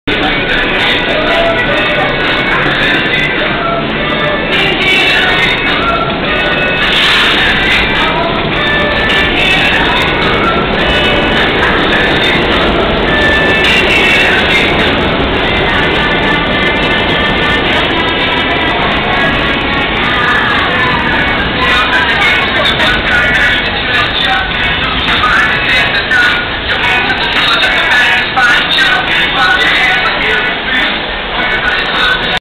R68A (B) Train At W 59th Street-Columbus Circle
This is an MTA NYC Subway video of the R68A (B) Train At W 59th Street-Columbus Circle heading Uptown.